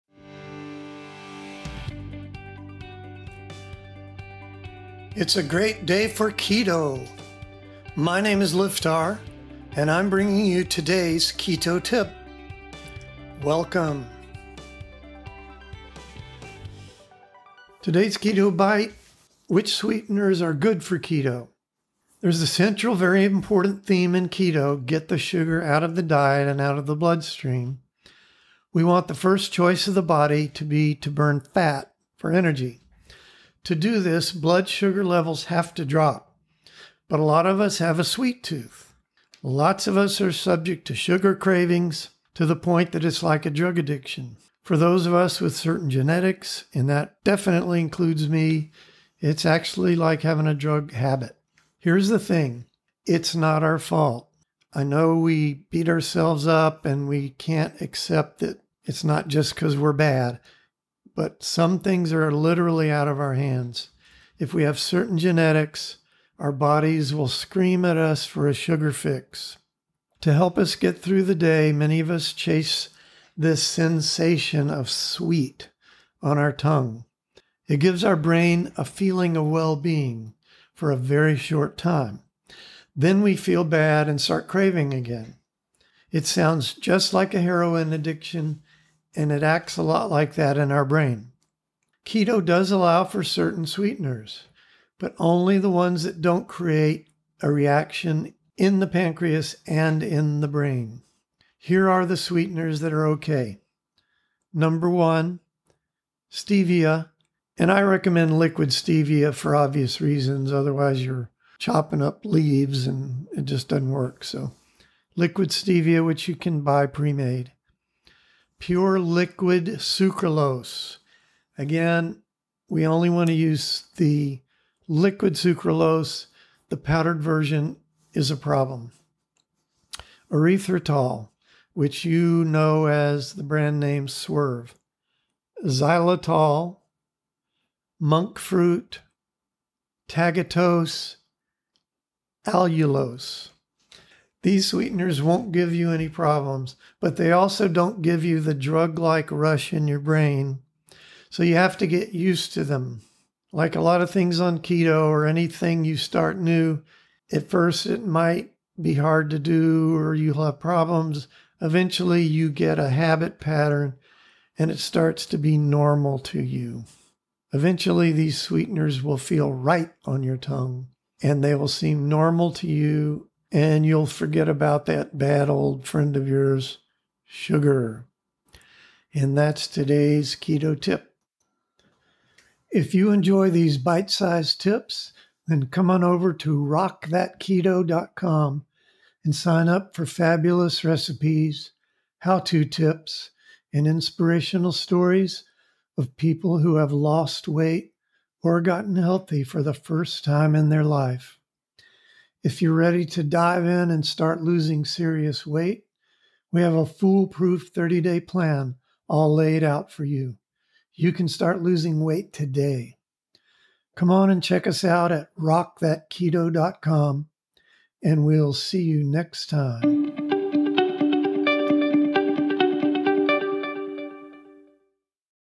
Keto Sweeteners that Are OK | Low Carb Sweeteners That Are Keto Safe 035
Good Sweeteners for Keto

(If you would like to get more….more free recipes, tips, advice, inspiration, and support, come sign up for our free emails

There is a central, very important theme in Keto...get the sugar out of the diet, and out of the blood stream.  We want the first choice of the body to be fat for energy.

To do this, the blood sugar levels have to drop.

But, a lot of us have a sweet tooth.

Many of us are subject to sugar cravings to the point that it’s like a drug addiction.  For those of us with certain genetics, and this includes me, it actually is exactly like a drug addiction.

Here’s the thing….It is not our fault. We humans like to beat ourselves up about everything. But this is literally out of our hands. If we have certain genetics, our bodies scream at us for a sugar fix.

To help us get through the day, many of us chase the sensation of “sweet” on our tongues. It gives our brain a feeling of well being...for a very short time. Then we feel bad, and the craving starts again.  It sounds a lot like a heroin addict.

 Keto does allow for certain sweeteners, but only the ones that don’t create a reaction in the pancreas, and a change in our blood chemistry.

Here are the sweeteners that are OK.

1. Stevia - I recommend Liquid Stevia
2. Pure Liquid Sucralose (liquid version only. powder is a problem)
3. Erythritol  (which you will find under the brand name Swerve)
4. Xylitol
5. Monk fruit
6. Tagatose
7. Allulose

These sweeteners won’t give you any problems, but they also don’t give you the drug like rush of sugar. You will have to get used to them. Everything on the Keto lifestyle gets better with time and repetition.

Your taste buds and your brain will adjust. After you have been off of sugar for a while, it won’t even taste good. These alternate sweeteners will start tasting “right” to your tongue.

And a quick recap from a previous article about which sweeteners to avoid.
Below are the sweeteners in the “Do Not Use” category.
This group is called the polyols.  If it has the ”tol” on the end, don’t use it.  When maltitol and other sugar alcohols get to large intestines, they get fermented by gut bacteria.  This creates  gas and bloating.
When you get  these unabsorbed sugars in the intestines, they attract water. This can cause diarrhea and or loose stools. If you already have some gut problem, like irritable bowel syndrome, you will be extra sensitive to these sugar alcohols. Anyway, just the name “sugar alcohols” should tell you it’s not Keto Kosher.
Here are the Polyol no no’s, plus others that don’t work for Keto.
1. Sorbitol
2. Maltitol
3. Splenda

4. Maltodextrin (which is also used as a pesticide)

5. Vegetable glycerin

6. Honey

7. Agave

8. Molasses

Get your body in the habit of these healthy sweeteners, and you’ll forget all about that nasty old drug habit you had…..Sugar!

 So Keep Up!  You can do this.

If you like these tips and would like to get more tips, endless recipes, and lots of inspiration, then come to our free website and sign up for our mailing list.
Act! Don’t React!
Have a Happy, Healthy Day
Livtar